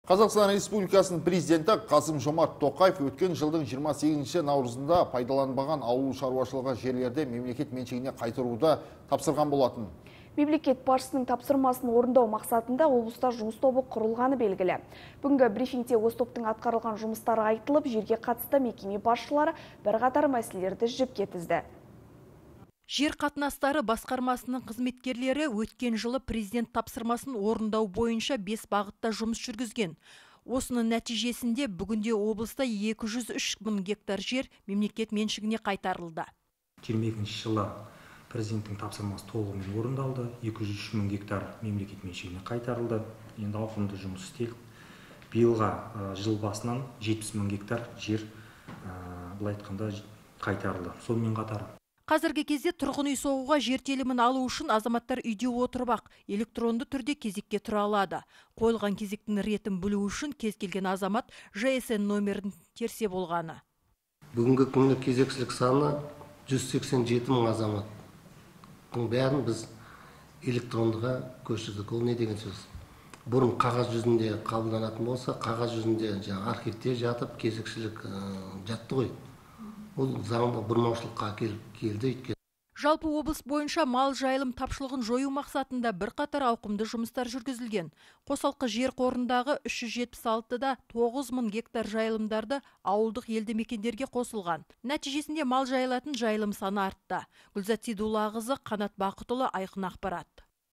Жер мәселесі - басты назарда
ҚР Президенті Қасым- Жомарт Тоқаев өткен жылдың 28 наурызында  пайдаланбаған ауылшаруашылығы жерлерді мемлекет меншігіне қайтаруды тапсырған болатын. Мемлекет басшысының тапсырмасын орындау мақсатында облыста жұмыс тобы құрылғаны белгілі. Бүгінгі брифингте осы топтың атқарылған жұмыстары айтылып, жерге қатысты мекеме басшылары бірқатар мәселелерді жіпке тізді.